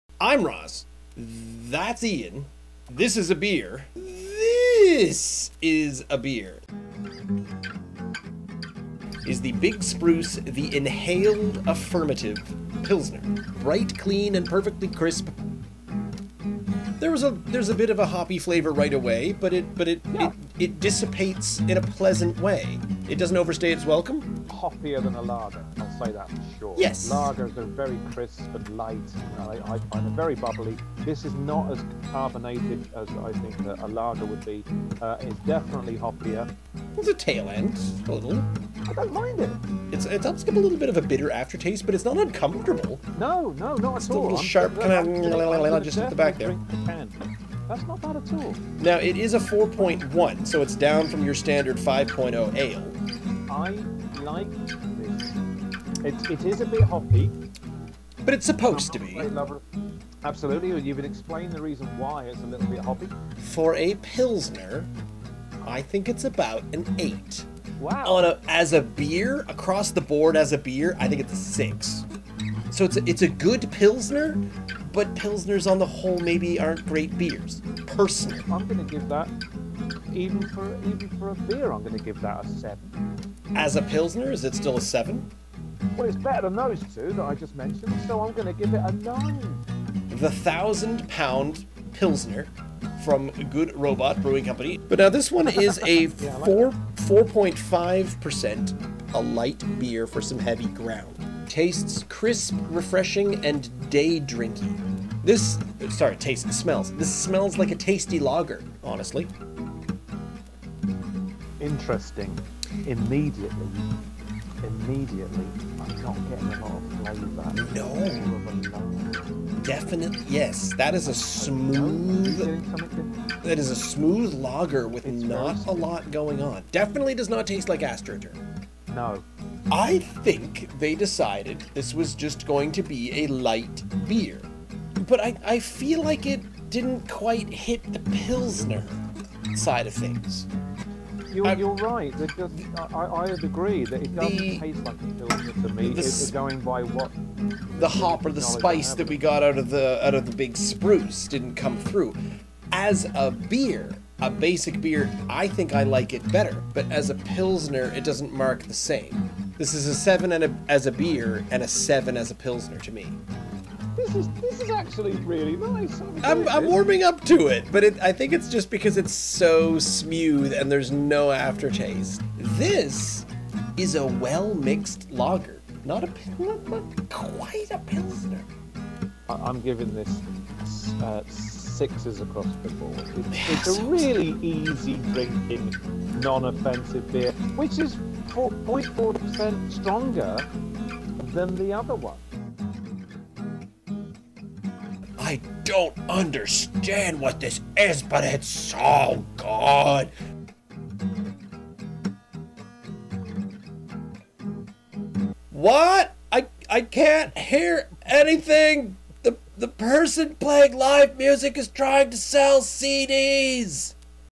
Ross & a Beer #50 - Big Spruce "Inhaled Affirmative" VS Good Robot "Ten Thousand Pound" Pilsner
Originally broadcast Feb 8, 2022, Ross and Ian try to figure out why a pilsner is its own beer style drinking Big Spruce's "Inhaled Affirmative" pilsner and Good Robot's "Ten Thousand Pound" pilsner.